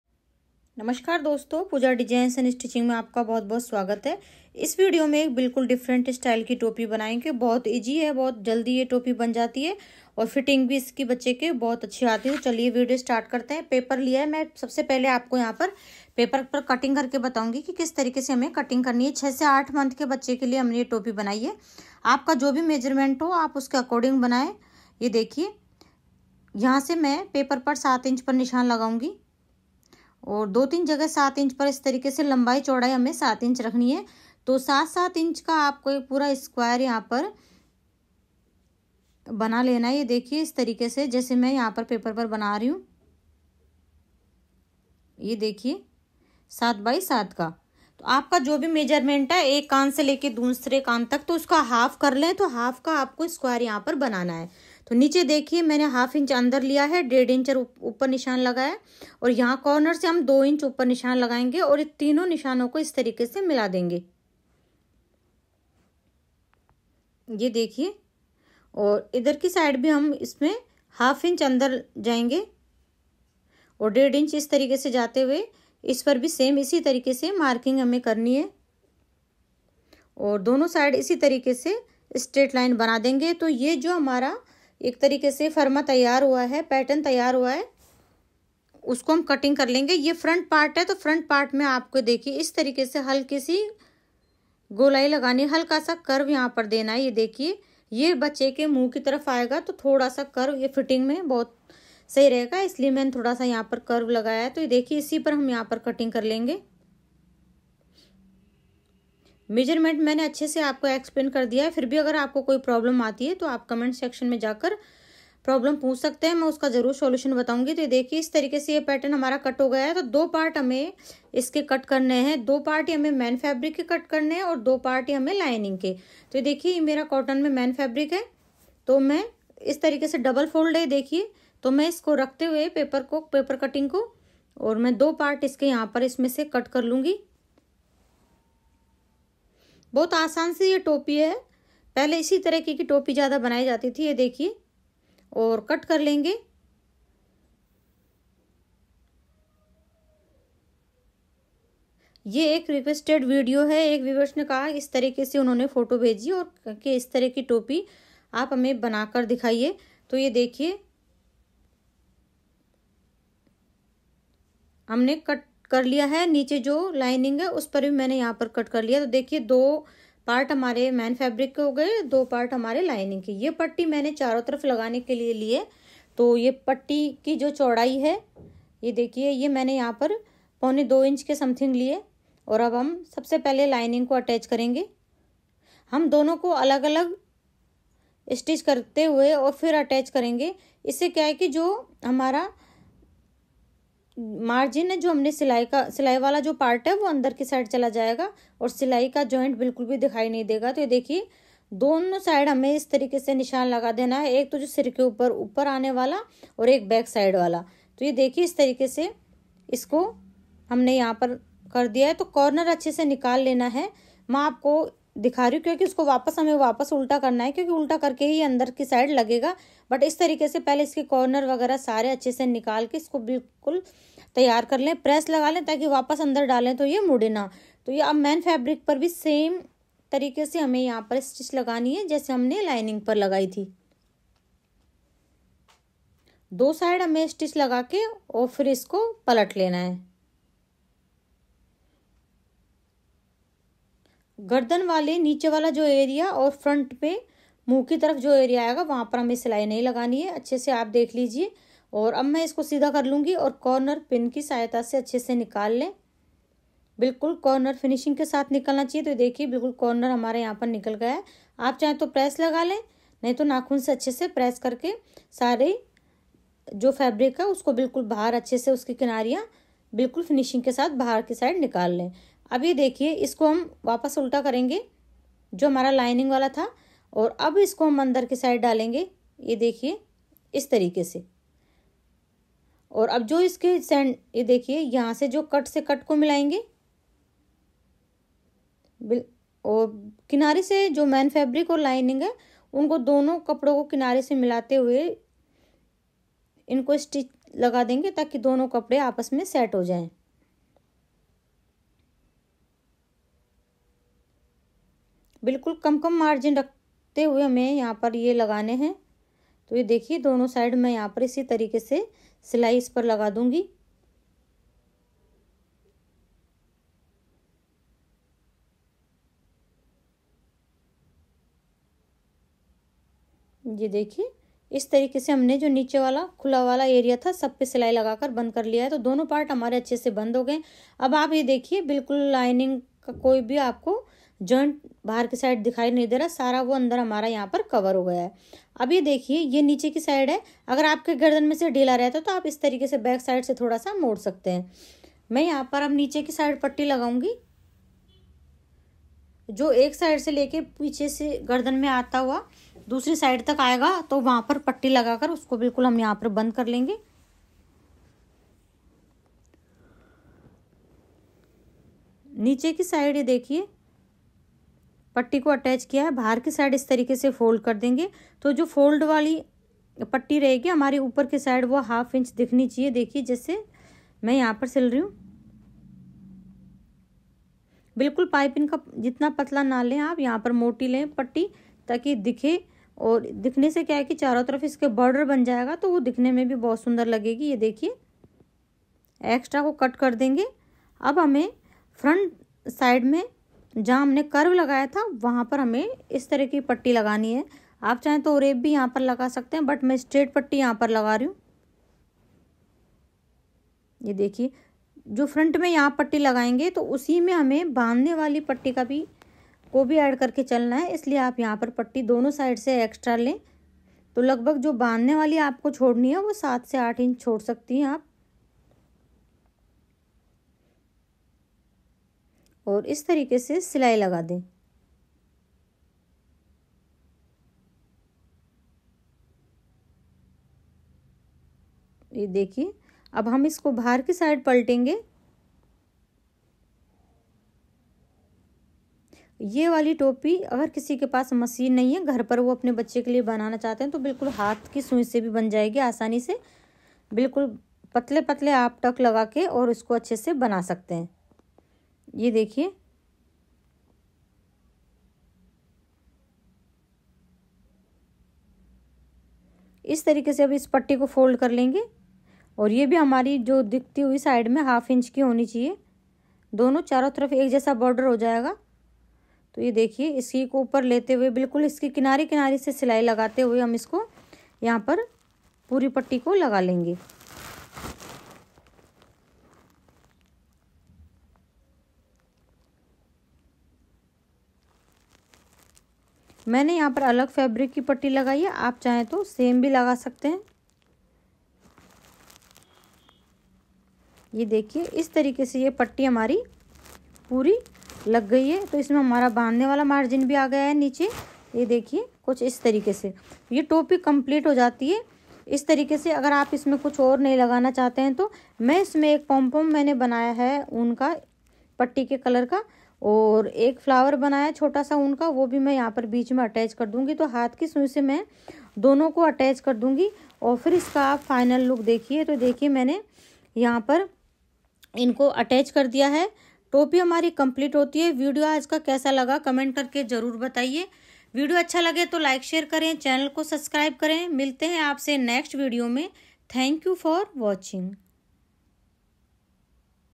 How to make newborn baby cap II Topi cutting and stitching II Bacho ki topi Banane ka Tarika II Topi
5 मिनट में आसान तरीके से बनाएं बेबी कैप (0 to 3 months)how to make newborn baby cap in just 5 minute

Hello Everyone,